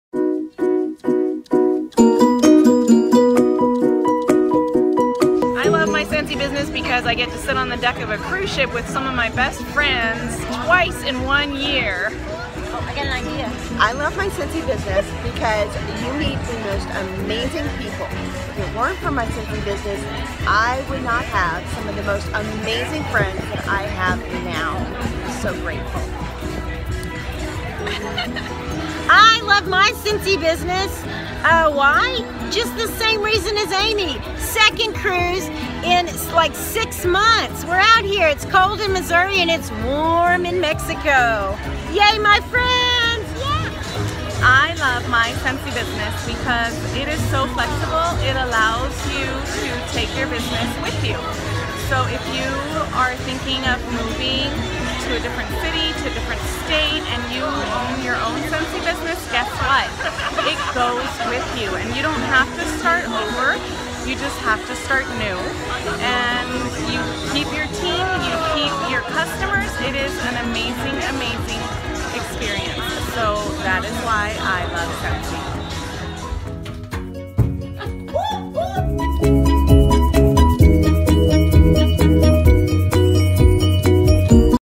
We Love Our Scentsy Business Because...
Flexible hours, incentive trips, good friends, family time...the list goes on! Just some of the reasons we love our Scentsy business!

For more information about how to join my team and become an Independent Scentsy Consultant, please